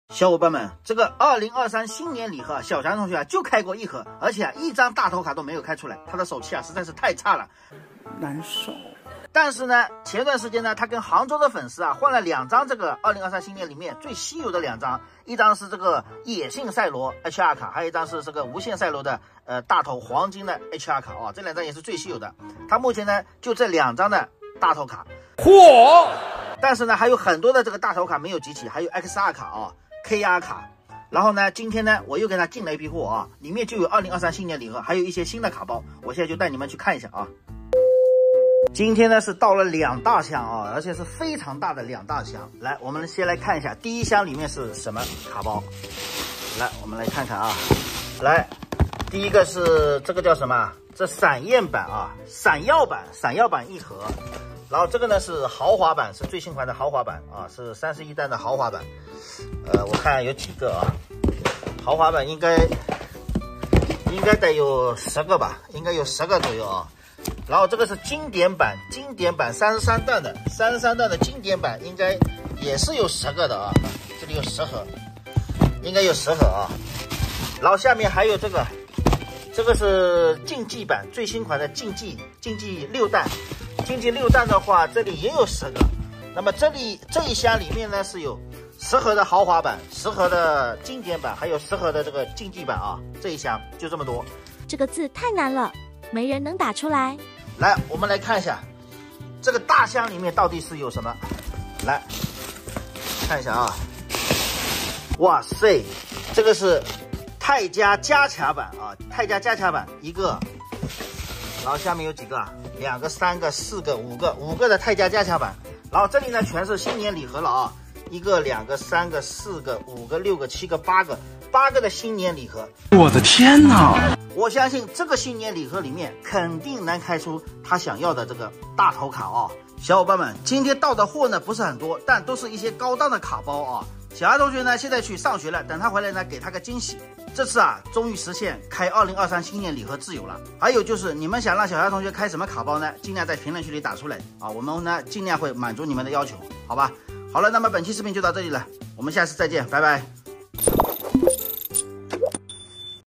新年礼盒没开够小翔很难过！老爸又进两大箱货，终于实现开卡自由【淘气包小翔同学】
新年礼盒没开够小翔很难过！
哈喽，大家好，我是淘气包小翔同学